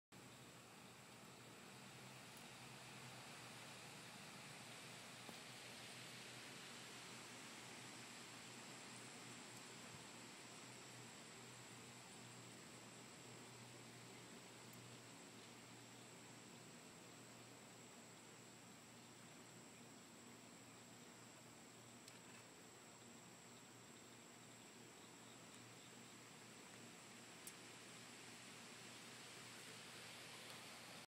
Meerkat Eats Couch.MOV
Stray kitten eating pieces of my old couch.